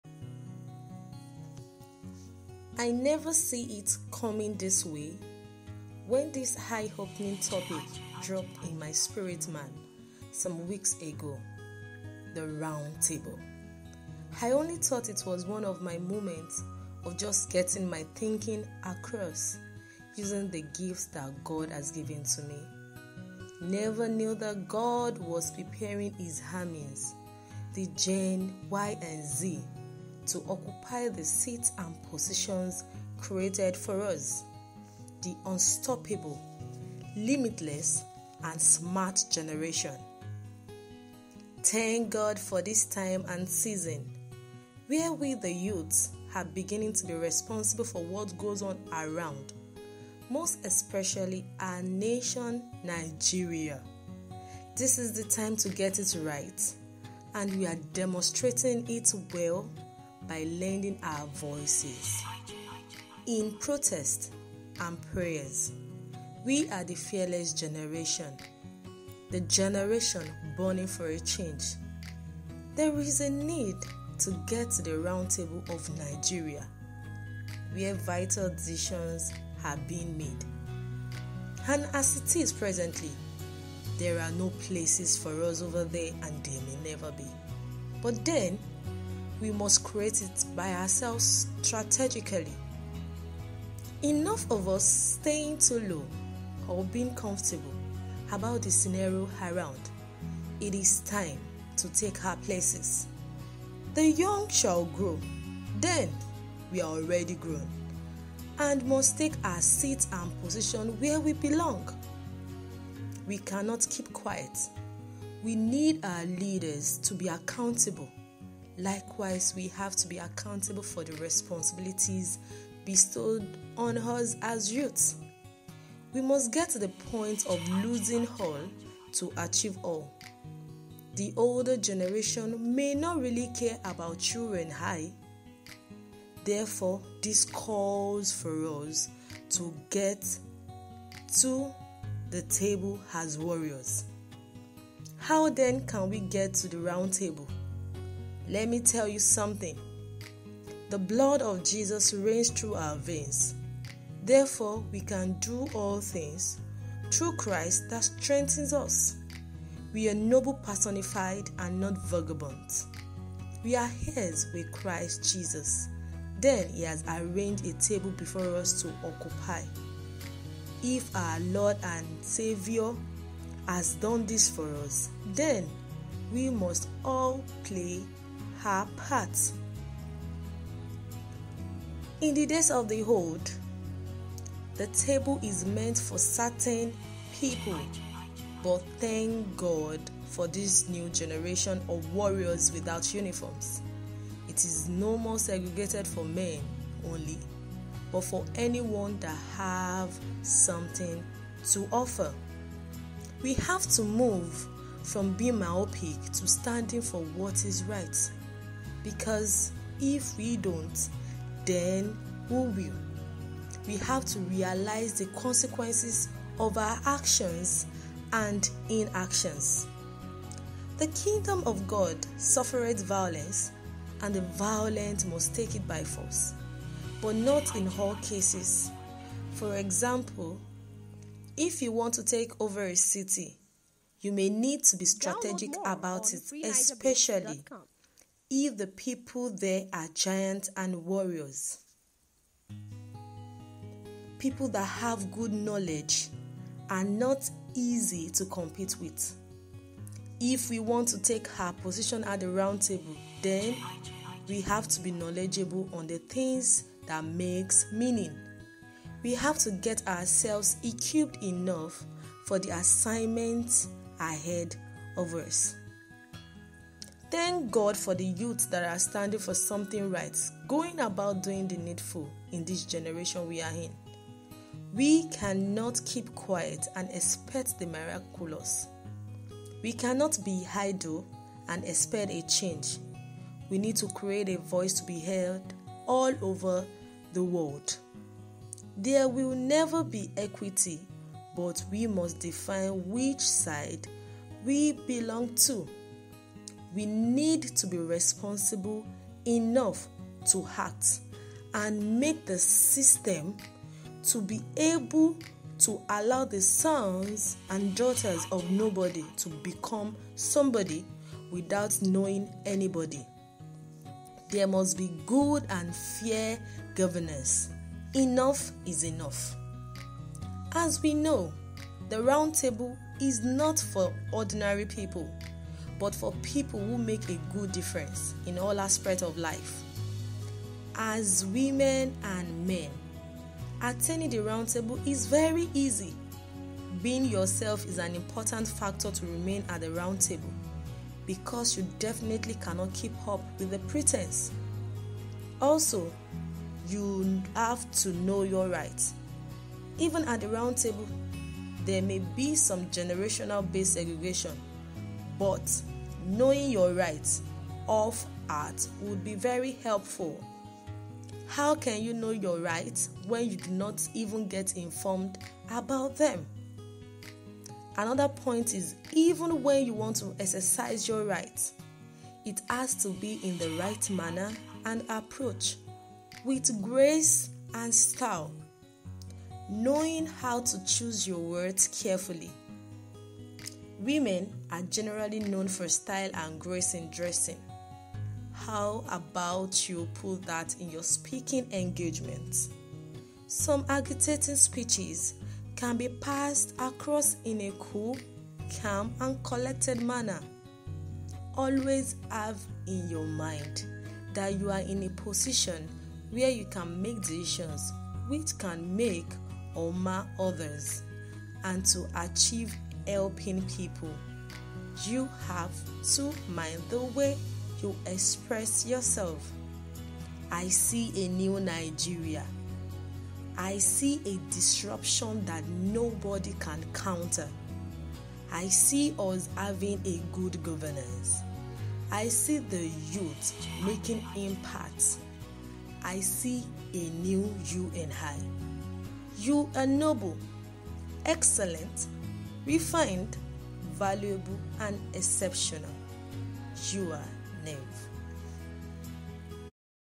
The Round Table (made with Spreaker)
Attaining and maintaining your seats at the Round table is very important as Youths. Having the right information and a good mannerism will help in achieving this.